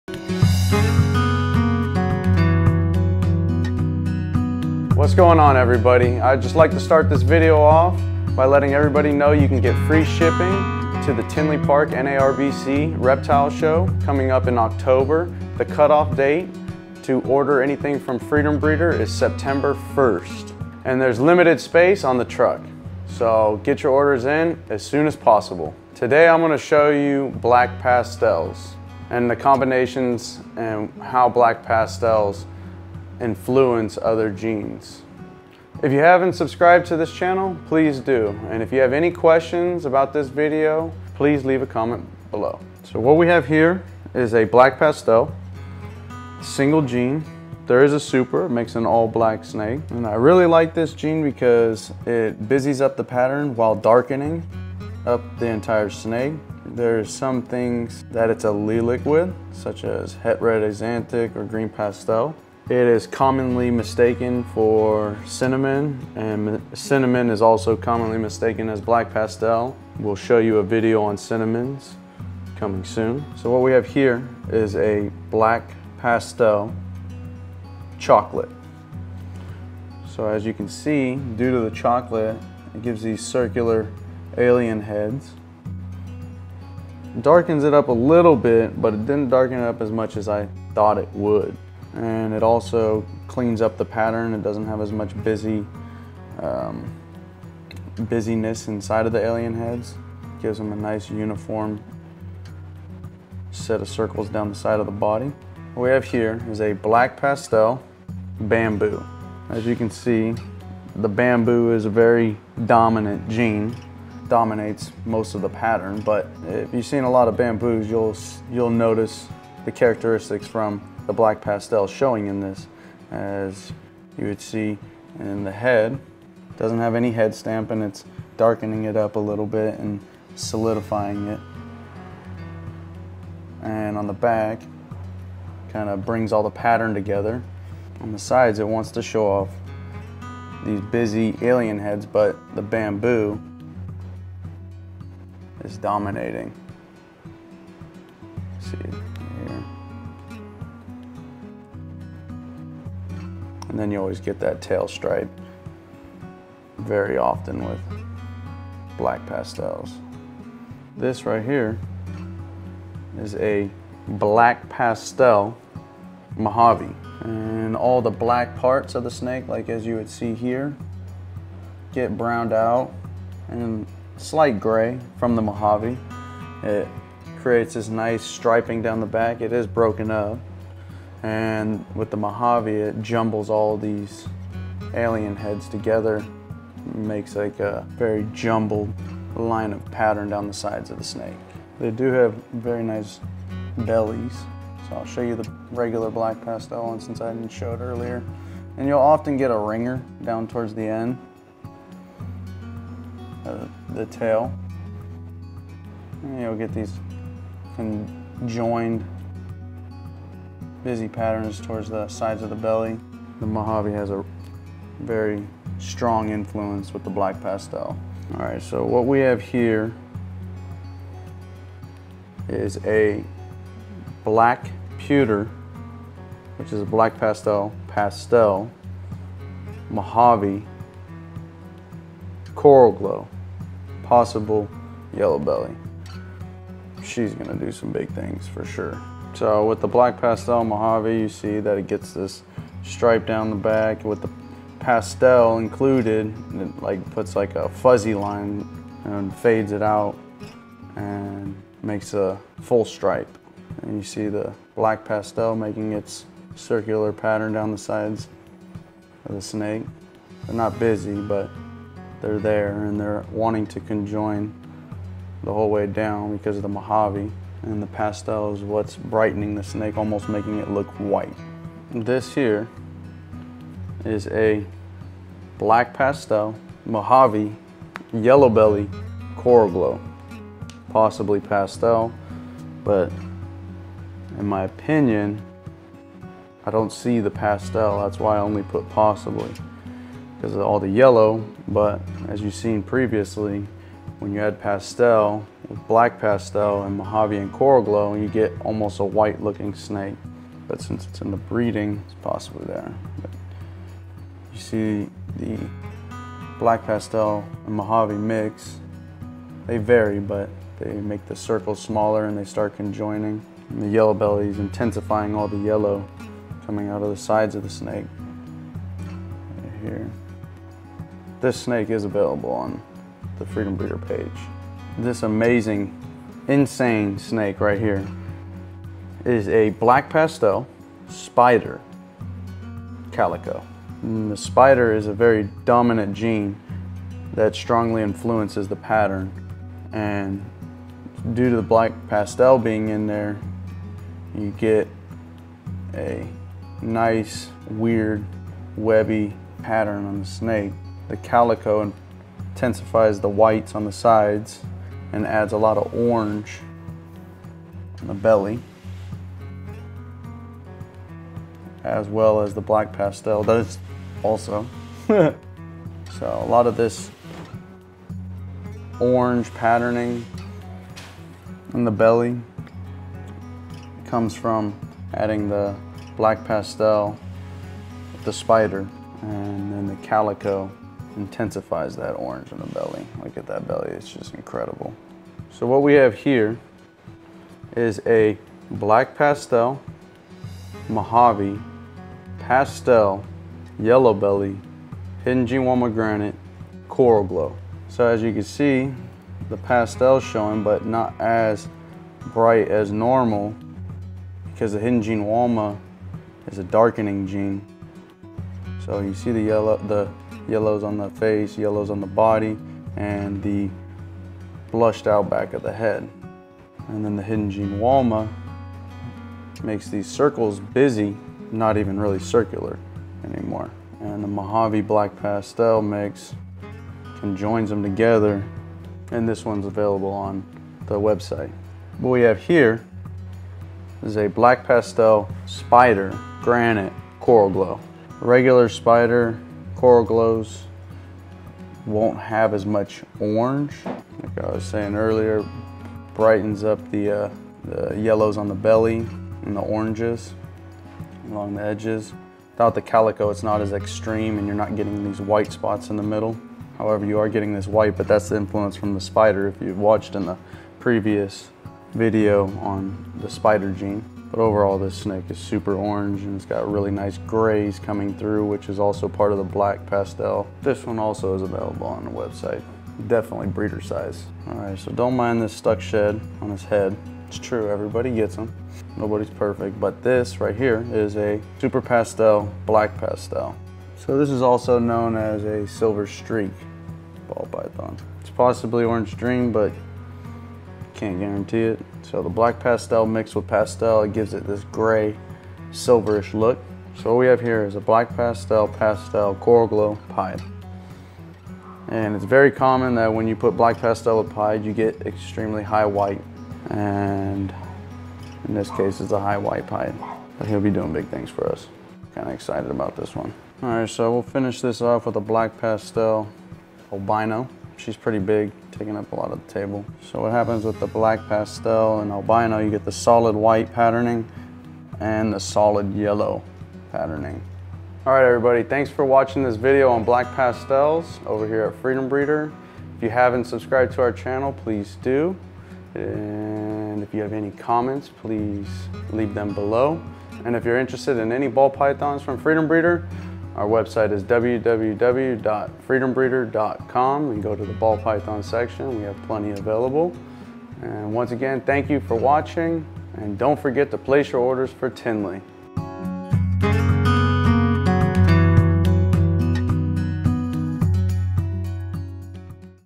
How to Identify Black Pastel in Ball Python Combos
In this video we're going to show you some of the markers to look for when trying to identify the black pastel gene in your ball python morph combos.